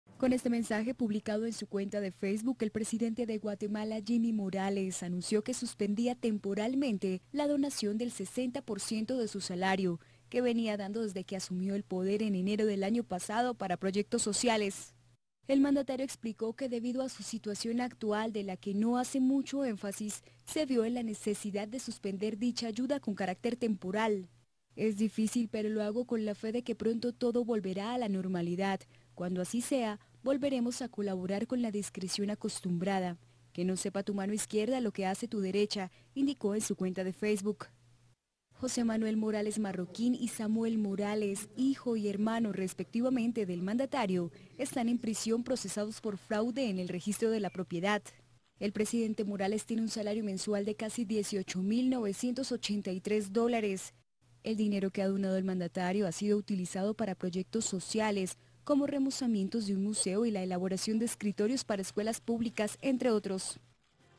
Presidente de Guatemala cancela temporalmente la donación del 60% de su sueldo
El Presidente de Guatemala, Jimmy Morales, suspendió temporalmente la donación del 60 % de su salario que venía dando desde que asumió la presidencia, así lo anunció a través de sus redes sociales.

EFE / CB24